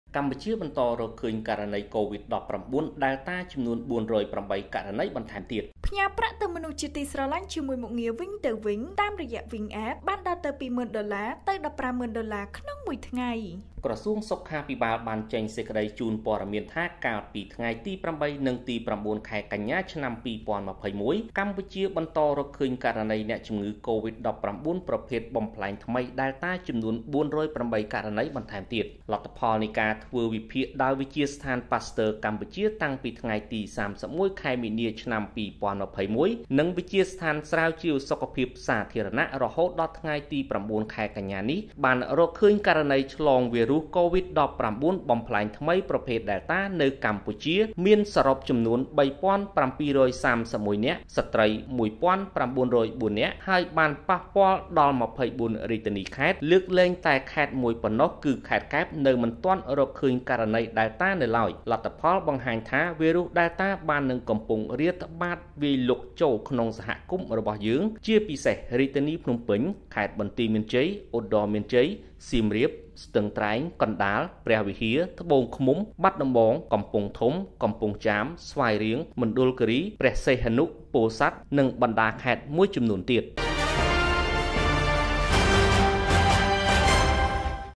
កម្ពុជា បន្តរកឃើញករណីកូវីដ១៩ប្រភេទ Delta ចំនួន ៤០៨ករណីបន្ថែមទៀត
ភ្នំពេញ៖ ក្រសួងសុខាភិបាល បានចេញសេចក្ដីជូនព័ត៌មានថា កាលពីថ្ងៃទី៨ និងថ្ងៃទី៩ ខែកញ្ញា ឆ្នាំ២០២១ កម្ពុជា បន្តរកឃើញករណីអ្នកជំងឺកូវីដ១៩ ប្រភេទបំប្លែងថ្មី Delta ចំនួន ៤០៨ ករណីបន្ថែមទៀត។